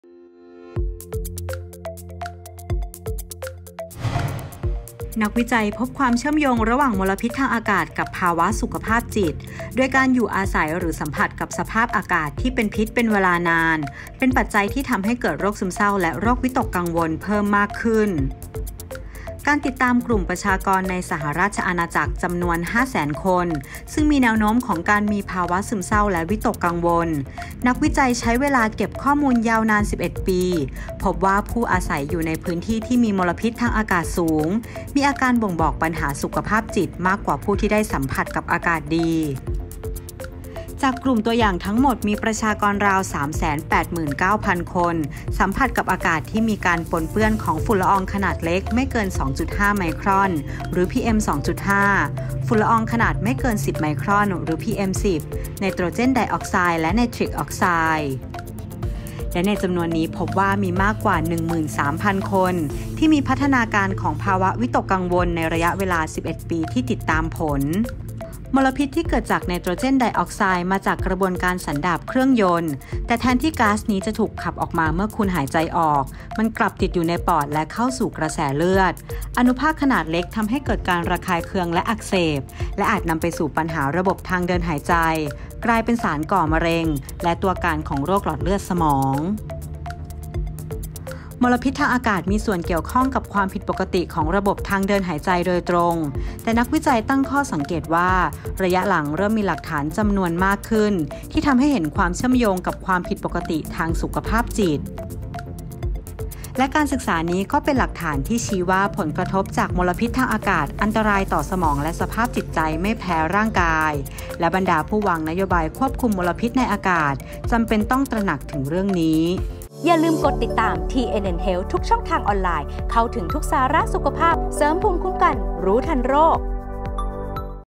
คุณภาพอากาศต่ำ ทำให้เกิดภาวะซึมเศร้าได้ | TNN HEALTH
นักวิจัยพบความเชื่อมโยงระหว่างมลพิษทางอากาศกับภาวะสุขภาพจิต โดยการอยู่อาศัยหรือสัมผัสกับสภาพอากาศที่เป็นพิษเป็นเวลานาน เป็นปัจจัยที่ทำให้เกิดโรคซึมเศร้าและโรควิตกกังวลเพิ่มมากขึ้น

การติดตามกลุ่มประชากรในสหราชอาณาจักรจำนวน 500,000 คนซึ่งมีแนวโน้มของการมีภาวะซึมเศร้าและวิตกกังวล นักวิจัยใช้เวลาเก็บข้อมูลยาวนาน 11 ปี พบว่า ผู้อาศัยอยู่ในพื้นที่ที่มีมลพิษทางอากาศสูงเกินเกณฑ์มาตรฐาน มีอาการบ่งบอกปัญหาทางสุขภาพจิตมากกว่าผู้ที่ได้สัมผัสอากาศดี